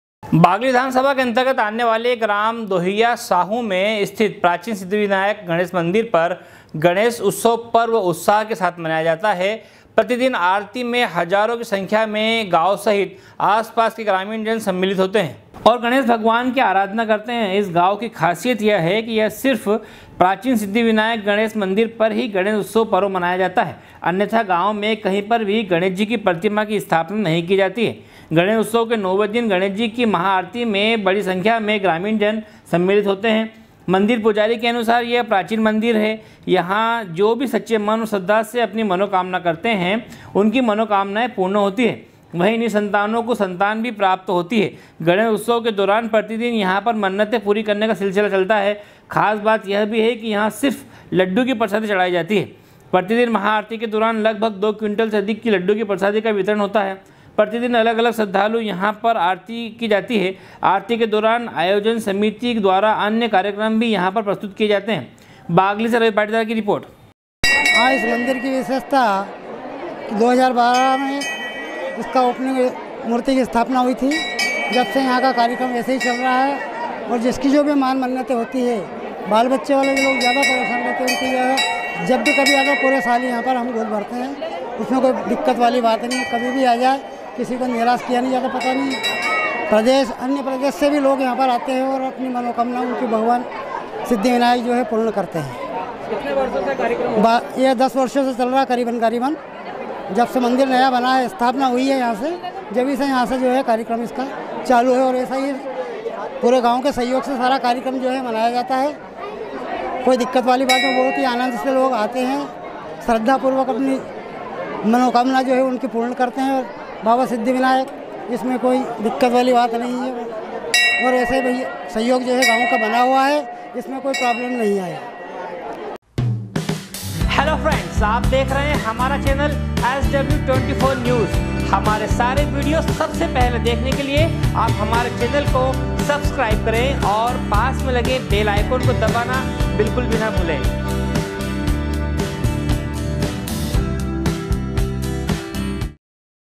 बागली के ग्राम देहरिया साहू में स्थित प्राचीन सिद्धिविनायक गणेश मंदिर पर गणेश उत्सव पर्व मनाया
बागली विकासखंड के अंतर्गत आने वाले ग्राम देहरिया साहू में स्थित प्राचीन सिद्धिविनायक गणेश मंदिर पर गणेश उत्सव पर्व उत्साह के साथ मनाया जाता है। प्रतिदिन आरती में हजारों की संख्या में गांव सहित आसपास से ग्रामीण जन सम्मिलित होते हैं, बगली से रवि पाटीदार की रिपोर्ट